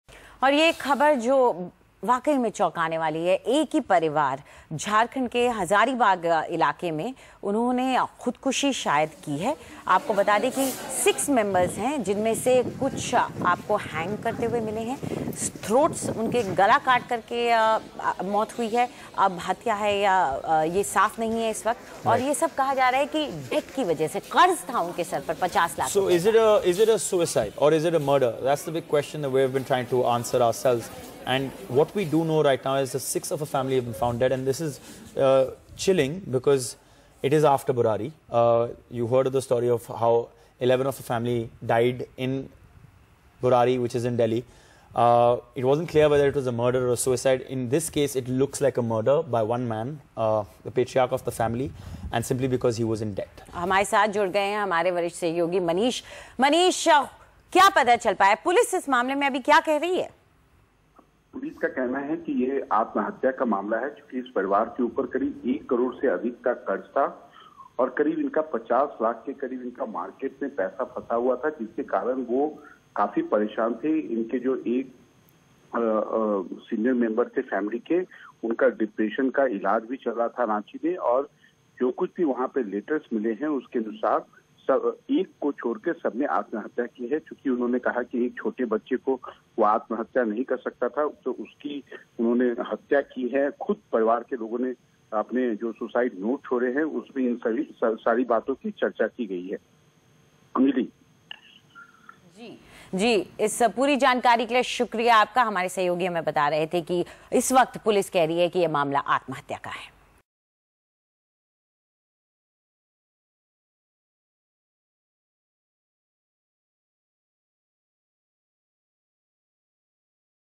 झारखंड के हजारीबाग में एक ही परिवार के 6 लोगों के शव मिले
झारखंड के हजारीबाग जिले से एक दिल दहला देने वाली घटना सामने आई है. शनिवार देर रात कर्ज में डूबे एक ही परिवार के छह सदस्यों की मौत से पूरा जिला सन्न रह गया. शुरुआती जांच में मौत की वजह कर्ज को बताई जा रही है, लेकिन परिवार की हत्या की आशंका से भी इंकार नहीं किया जा रहा है.